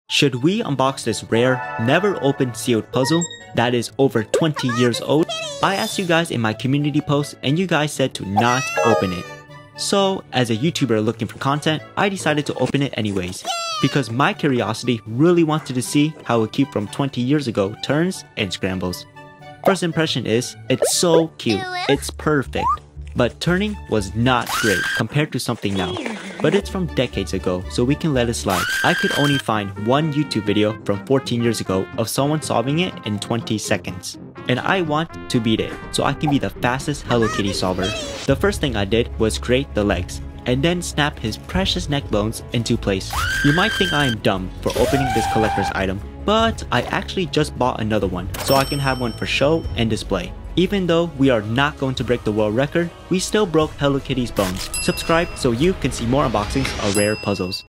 I made it scream…
Today I got this rare Hello Kitty 2x2 Rubik’s cube that is over 20 years old!